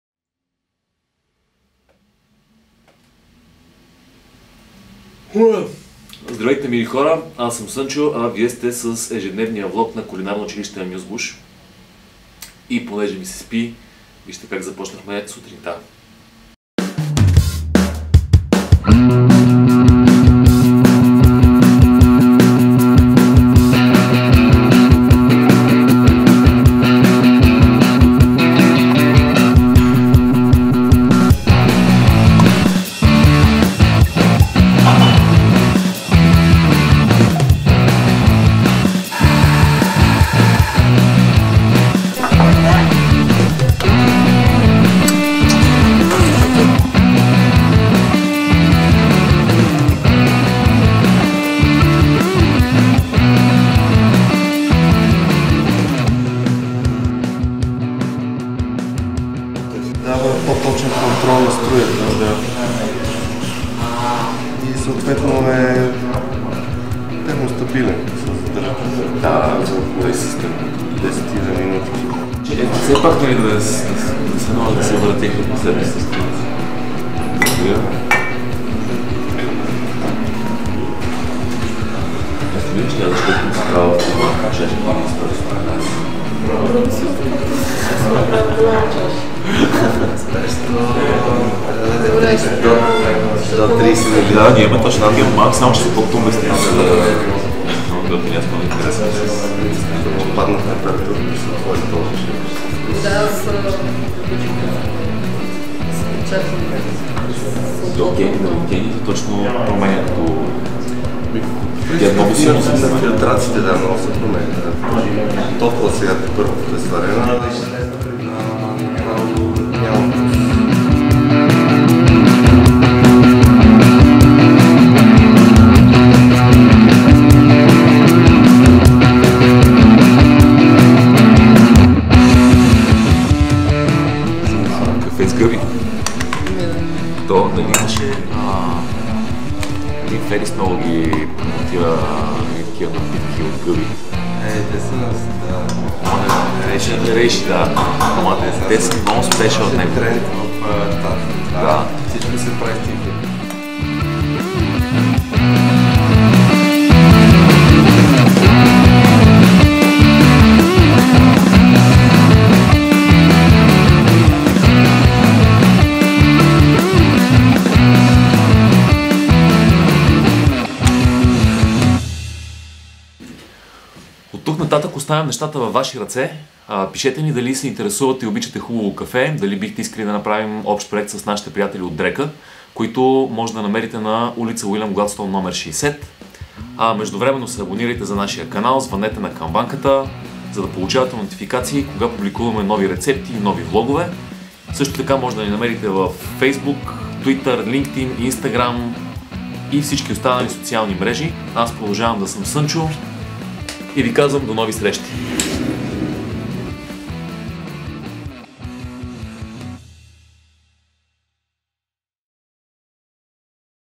When you feel sleepy it’s time for a hot brew
Today we went to taste some great coffee and discuss some future projects. By the end of the meeting sleep was the last thing on our mind. If you have not tried Drekka's coffee yet, it is time to correct that ,mistake. And one more thing ... there will be more news from our meeting soon.